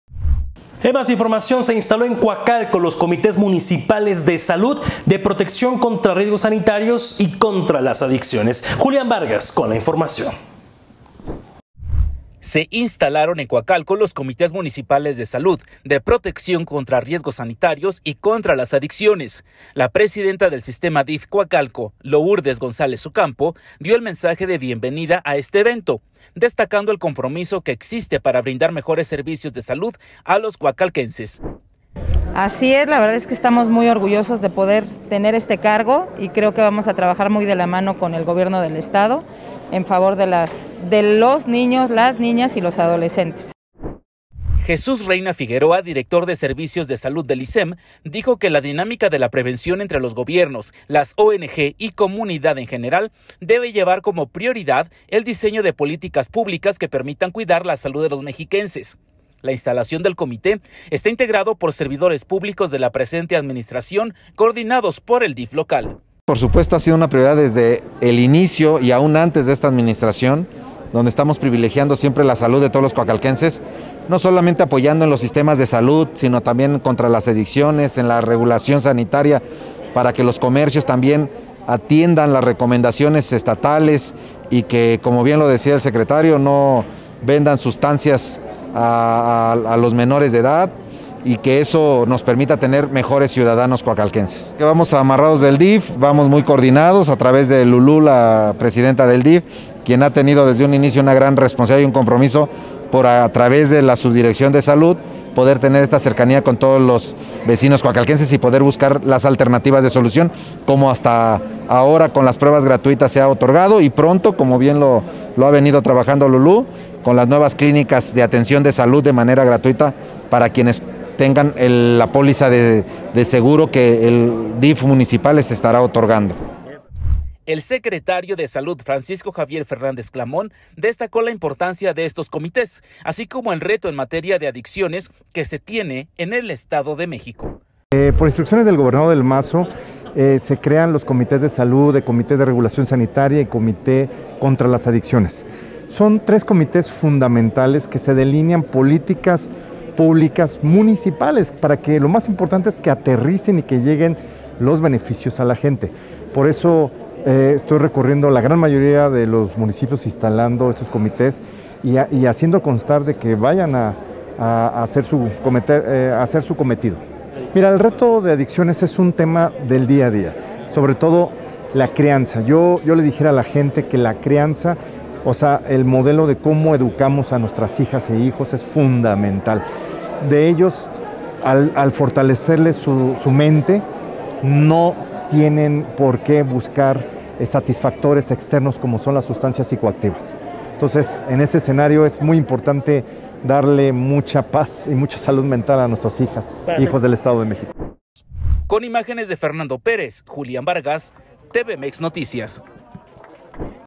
Se instaló en Coacalco los Comités Municipales de Salud, De protección contra riesgos sanitarios
Se instaló en Coacalco los Comités Municipales de Salud, De protección contra riesgos sanitarios y contra las adicciones

La Presidenta del Sistema Dif Coacalco dio el mensaje de bienvenida a este evento, destacando el compromiso que existe para brindar mejores servicios de salud a los coacalquenses.

Jesús Reyna Figueroa Director de servicios de salud del Isem Dijo que la dinámica de la prevención entre los gobiernos, las ong y comunidad en general debe llevar como prioridad el diseño de políticas públicas que permitan cuidar la salud de los mexiquenses.

La instalación del comité está integrado por servidores públicos de la presente administración coordinados por el Dif local

El Secretario de Salud Francisco Javier Fernández Clamont destacó la importancia de estos comités así como el reto en materia de adiciones que se tiene en el Estado de México